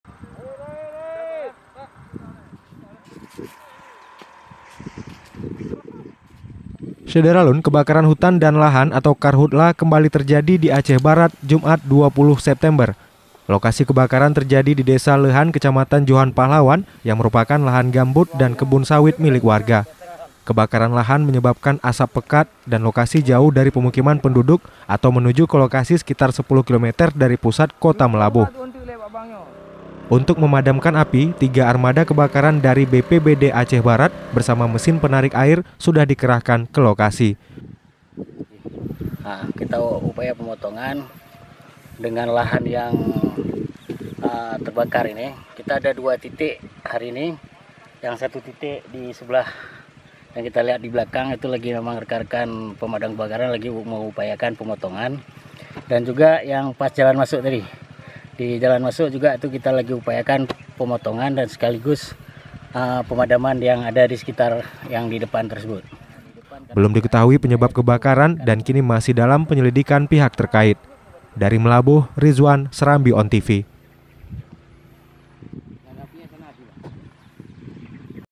Lahan Gambut di Aceh Barat Kembali Terbakar
SERAMBITV.COM, MEULABOH - Kebakaran hutan dan lahan (karhutla) kembali terjadi di Aceh Barat, Jumat (20/9/2019).

Lokasi kebakaran terjadi di Desa Leuhan Kecamatan Johan Pahlawan yang merupakan lahan gambut dan kebun sawit milik warga.

Kebakaran lahan menyebabkan asap pekat dan lokasi jauh dari pemukiman penduduk atau menuju ke lokasi sekitar 10 kilometer dari pusat kota Meulaboh.

Untuk memadamkan api, tiga armada kebakaran dari BPBD Aceh Barat bersama mesin penarik air sudah dikerahkan ke lokasi.

Belum diketahui penyebab kebakaran dan kini masih dalam penyelidikan pihak terkait.